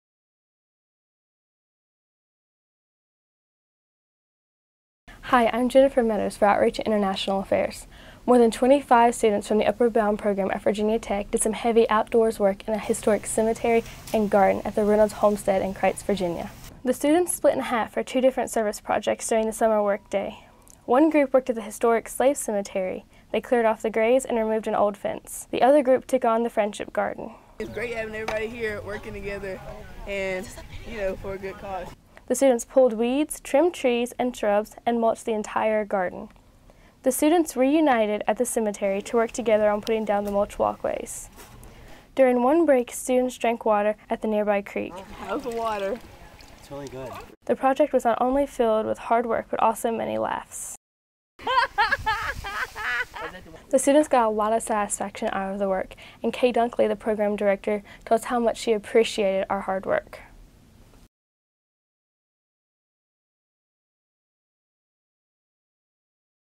Virginia Tech Upward Bound at Reynolds Homestead
A group of Upward Bound students from Virginia Tech spent a day working
on two projects at the Reynolds Homestead in Critz, Va.